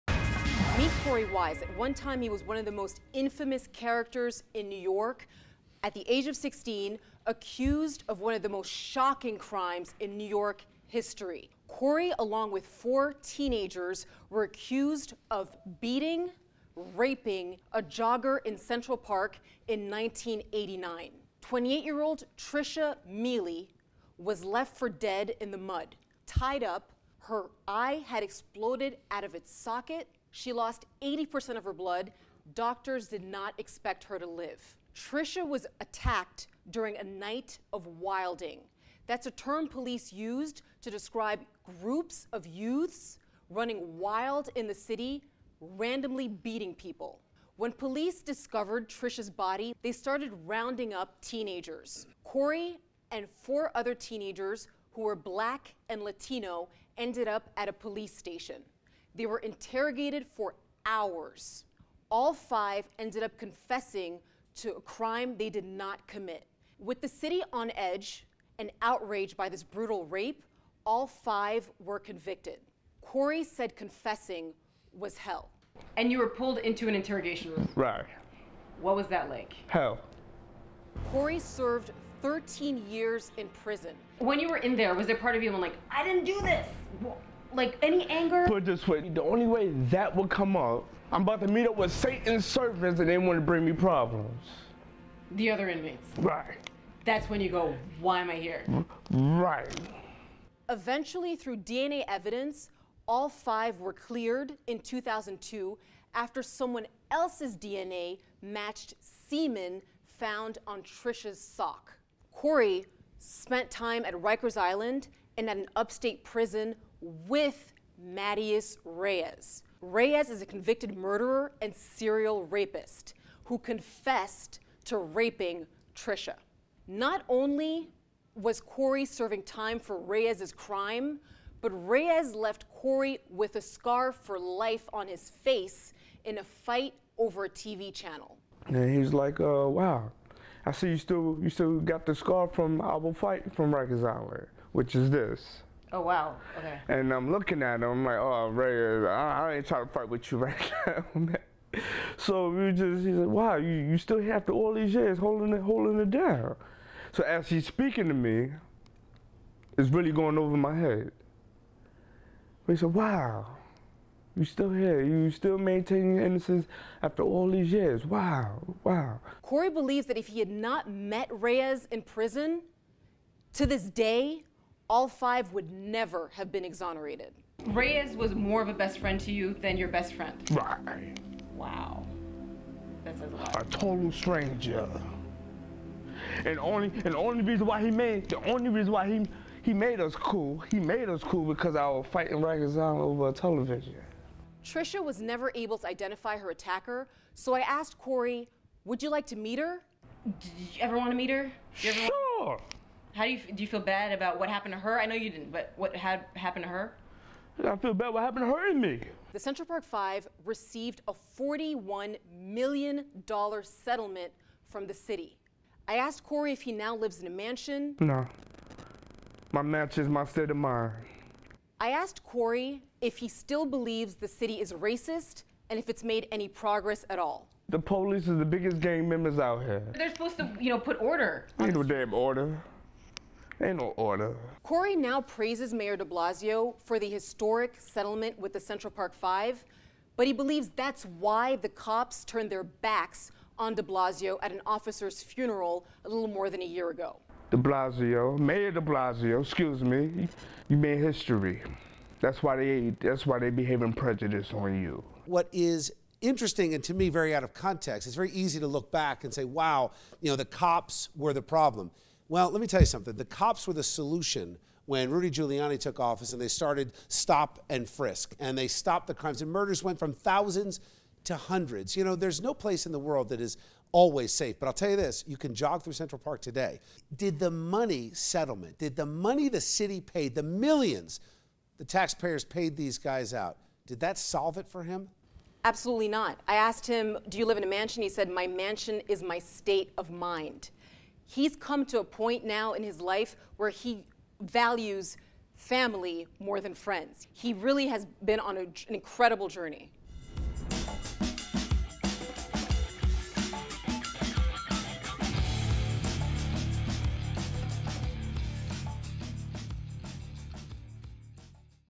One Wild Night Long Ago ...
Meet Korey Wise. At one time, he was among the most infamous characters in New York, accused at age 16 in one of the most shocking crimes in New York history: the case of the Central Park jogger.

We sat down for an exclusive interview with Korey, now 43, at one of his two homes in New York. S